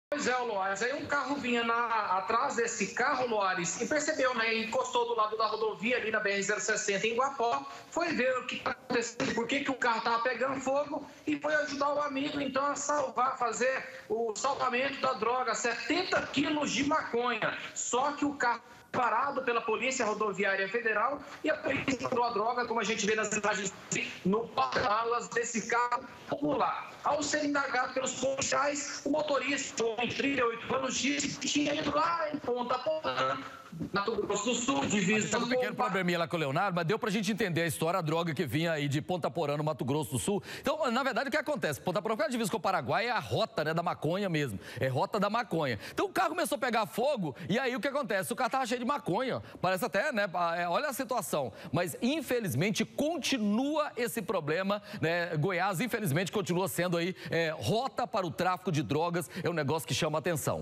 CASO INUSITADO - HOMEM “SALVA” DROGA DE CARRO PEGANDO FOGO E ACABA PRESO.
HOMEM “SALVA” DROGA DE CARRO PEGANDO FOGO E É PRESO PELA PRF EM GUAPÓ. LEONARDO GONÇALVES.